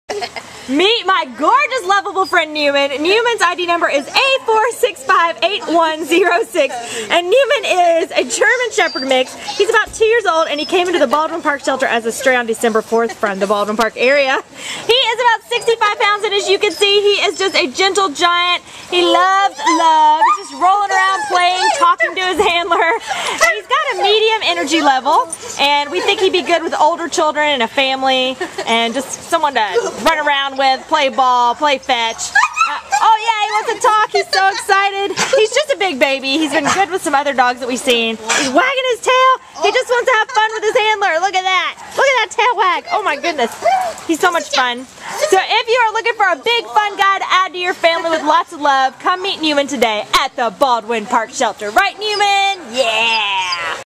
Newman A4658106
A4658106 Newman is a handsome two year old black and tan male German Shepherd mix who came to the Baldwin Park Animal Care Center as a stray from Baldwin Park on Dec. 4th with his sister, Nora [A4658102]. Weighing about 65 lbs, Newman walks capably on leash but he could benefit from some additional training. He gets along superbly with other dogs—especially larger, more mature dogs. He is a big baby and a gentle giant who is super affectionate with people and always has a big smile on his face. Newman will make a great indoor pet for any private household without cats, as he seems to be a bit excitable around the cats at the shelter.